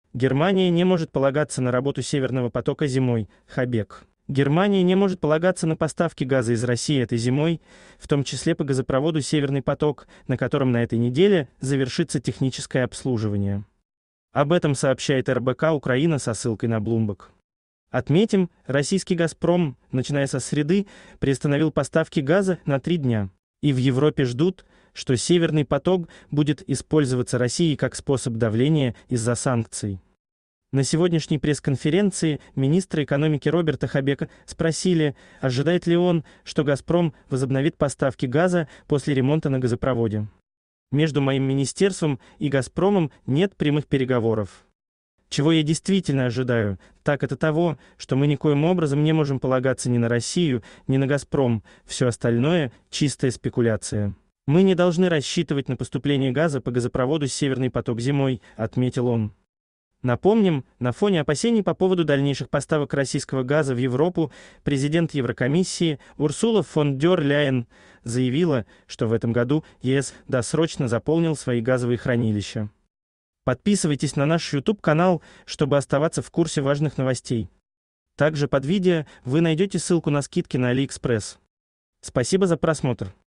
Германия не может полагаться на работу "Северного потока" зимой, - Хабек.
Германия Северныйпоток Газ Получи скидку на Алиэкспресс по этой ссылке до 70% + дополнительные скидочные купоны от продавцов. Германия не может полагаться на поставки газа из России этой зимой, в том числе по газопроводу "Северный поток", на котором на этой неделе завершится техническое обслуживание. Об этом сообщает РБК-Украина со ссылкой на Bloomberg. Отметим, российский "Газпром", начиная со среды, приостановил поставки газа на три дня. И в Европе ждут, что "Северный поток" будет использоваться Россией как способ давления из-за санкций. На сегодняшней пресс-конференции министра экономики Роберта Хабека спросили, ожидает ли он, что "Газпром" возобновит поставки газа после ремонта на газопроводе. "Между моим министерством и "Газпромом" нет прямых переговоров. Чего я действительно ожидаю, так это того, что мы никоим об